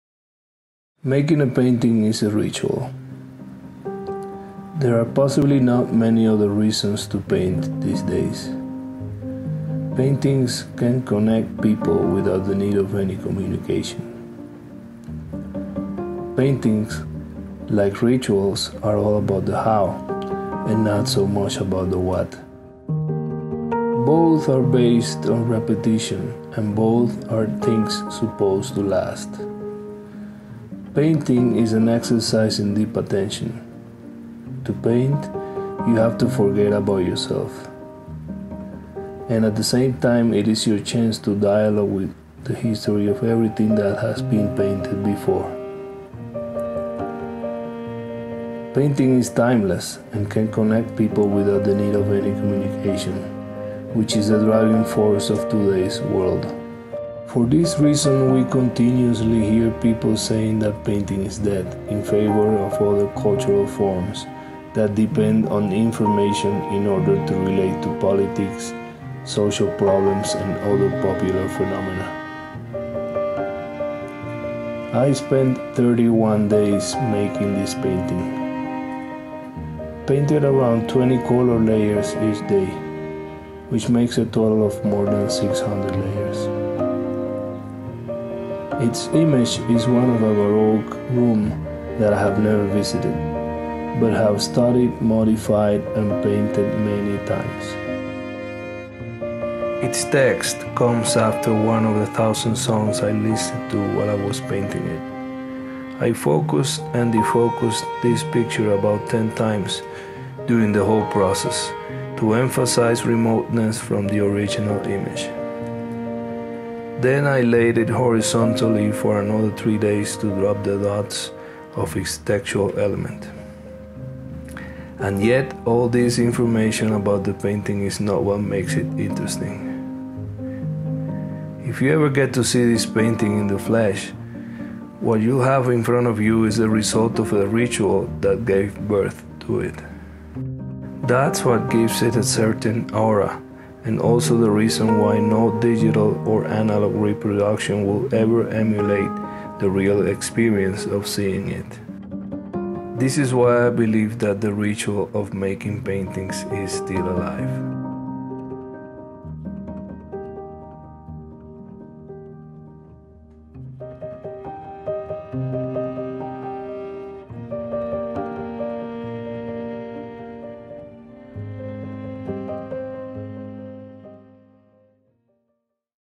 Raúl Cordero. BEHIND THE ARTWORK. Episode 3 "Painting As A Ritual"
In the third episode of BEHIND AN ARTWORK, Raúl Cordero explains the process of painting seen as a ritual, through the 31 days long process of of painting TODO EL MAMBO, TODO EL MUNDO (After Byrne), 2020.